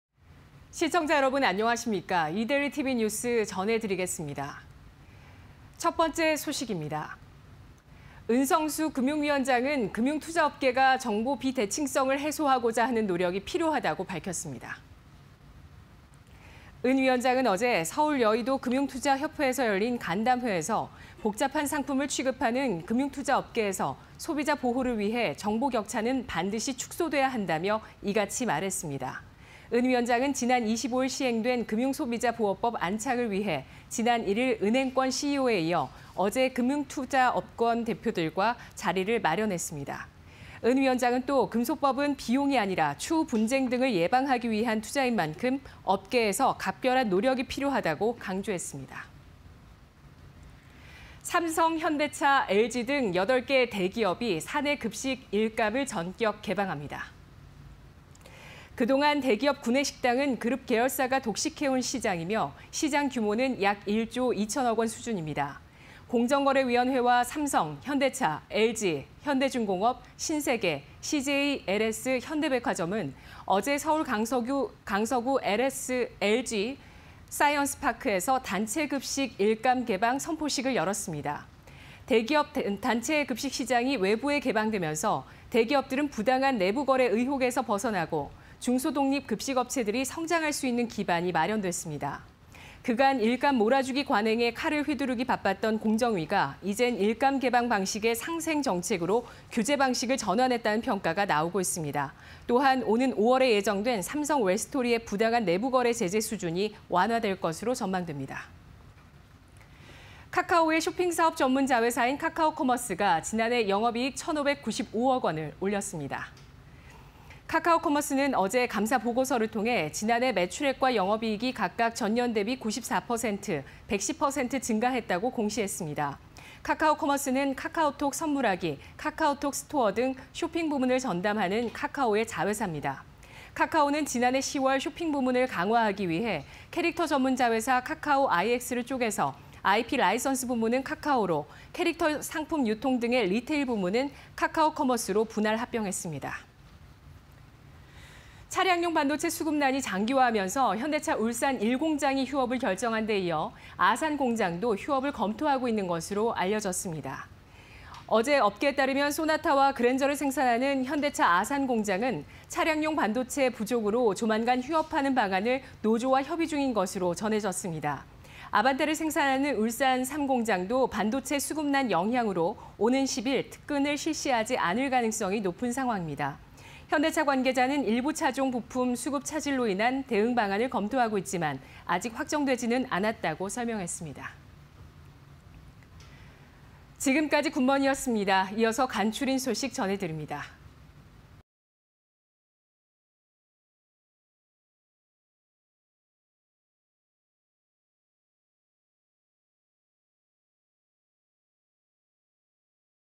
[이데일리N] 은성수 “금투업계, 정보 비대칭성 해소 노력 필요” 外 (20210406)
- 은성수 “금투업계, 정보 비대칭성 해소 노력 필요”
- 삼성·현대차·LG, 1.2조 구내식당 열린다
- 카카오커머스 작년 영업익 1595억… 110% 증가
- 차량용 반도체 수급난에... 현대차 아산공장도 휴업 검토

[간추린 소식]
- “유럽, 미래 전기차 배터리 강자… 2030년 31% 점유할 듯”
- 정세균 국무총리 내주 사의표명 가닥... 후임에 김부겸·이태복 거론
- 에스엠 “계열사 에스엠스튜디오스 주식 2446억원에 취득”
- 비트코인 등 가상화폐 시총, 사상 처음 2조 달러 돌파